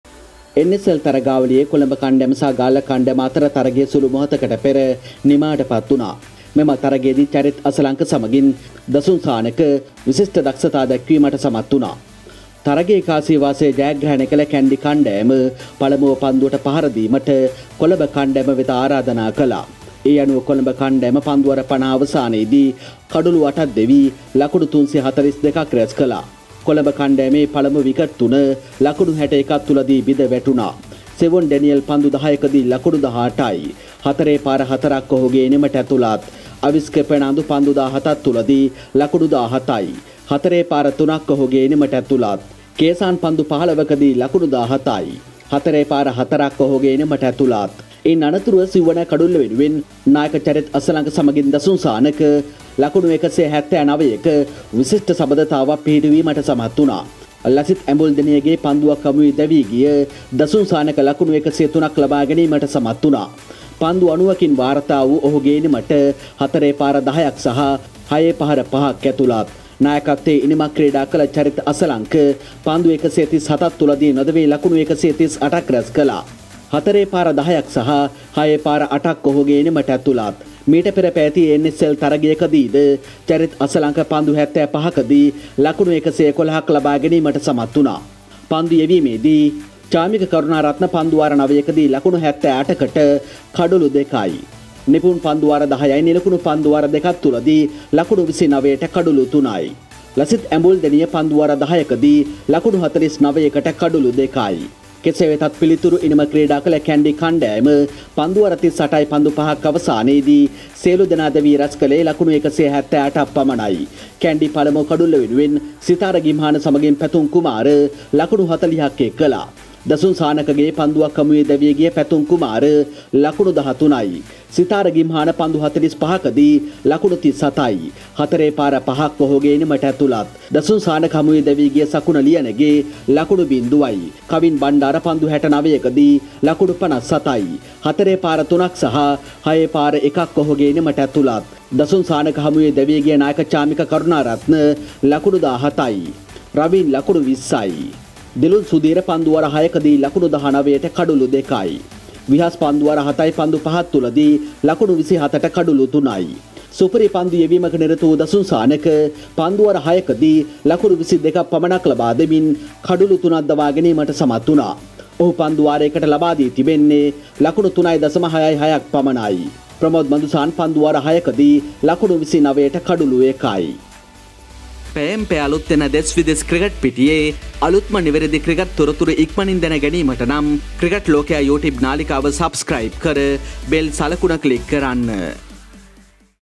දස්සා හා අසලංකගෙන් අති විශිෂ්ට ශතක දෙකක්,ඒ මැද්දේ දස්සගෙන් විශිෂ්ට පන්දු යැවීමකුත් / Cricket lookaya
ශ්‍රී ලංකාව ටෙස්ට් ශ්‍රේණිගතකිරීම්හී ඉදිරියටම,මිලාන්ගෙන් 48ක පිම්මක්,අසිත ලොව හොදම 10 අතරට